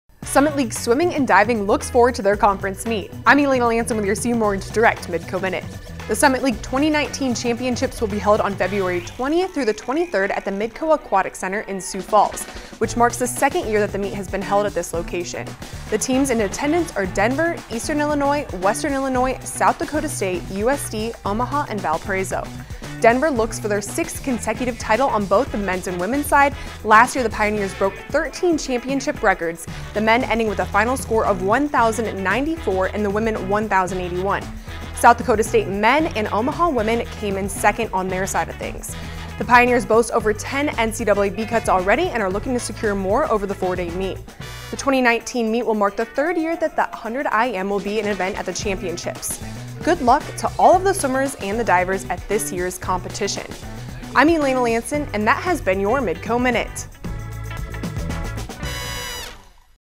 Midco Minute 516: Summit League S+D
Summit League Swimming and Diving looks forward to their conference meet.

The Summit League 2019 championships will be held on February 20 through the 23 at the Midco Aquatic Center in Sioux Falls, which marks the second year that the meet will be held at this location.

The teams that will be in attendance are Denver, Eastern Illinois, Western Illinois, South Dakota State, USD, Omaha, and Valparaiso.

Denver looks for their 6th consecutive league title on both the men's and women's side, last year the Pioneers broke 13 championship records. The Denver men ending with a final score of 1,094 and the women 1,081.  The South Dakota State men came in second, and the Omaha touched for second on the women's side of things. The pioneer’s boast over 10 NCAA B cuts already and are looking to secure more over the four day meet.

The 2019 meet will mark the 3rd year the 100 IM will be an event at the championships.

Good luck to all of the swimmers and divers in the competition.